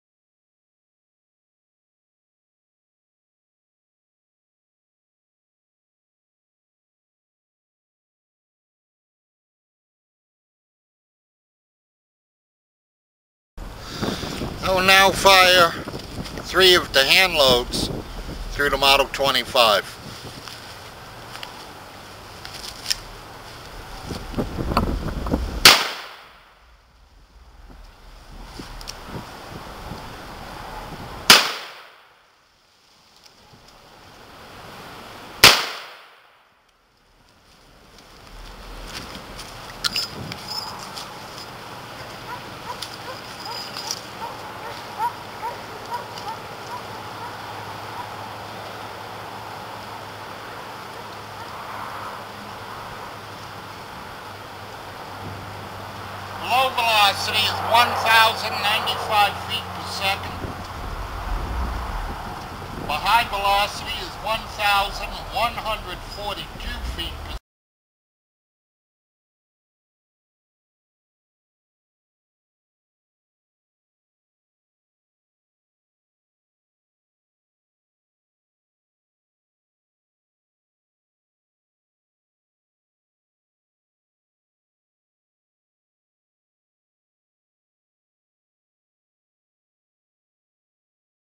45 Colt handload velocity test in a S&W M25-5
This is a continuation of the video in which I test a full load of 2400 behind a 250 grain XTP load in a  Winchester 94 Trapper Carbine.  In that weapon these cartridges achieve 1600 fps.  So let's see what velocity the same load achieves in a 4 inch Model 25-5.

I do not advocate this load for your guns.  Enjoy.